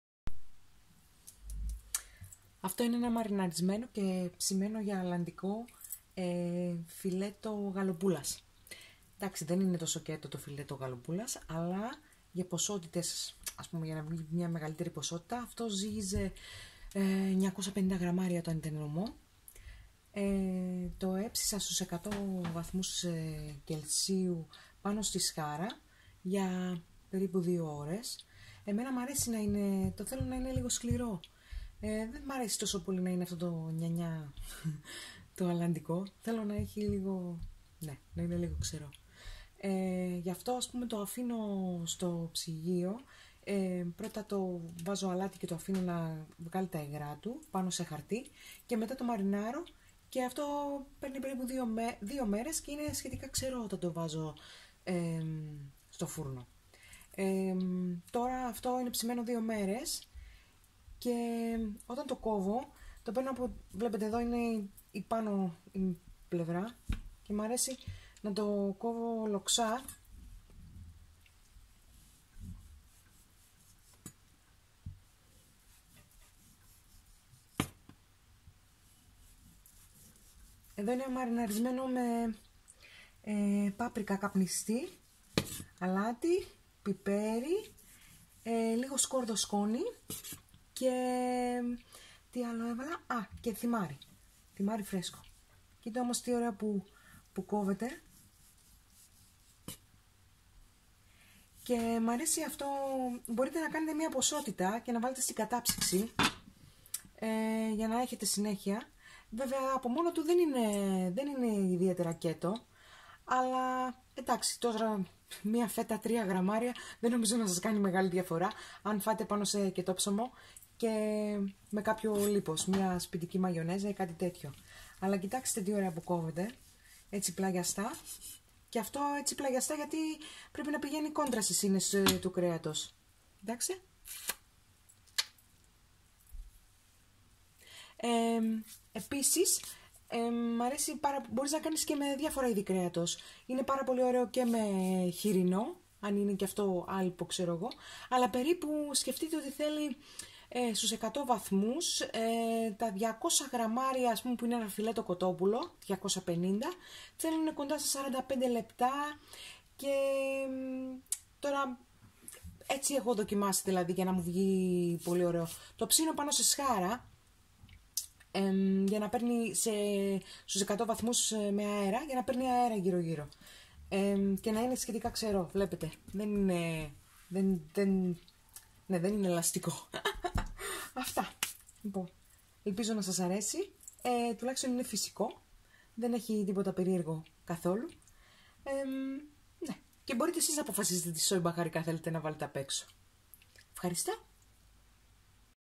Κόψιμο αλλαντικού, homemade.
Στο βίντεο είναι φιλέτο γαλοπούλας, δεν είναι ιδιαίτερα κέτο από μόνο του, αλλά δύο φέτες σε ψωμί κέτο με μπόλικο λίπος και βγαίνουν τα μάκρος!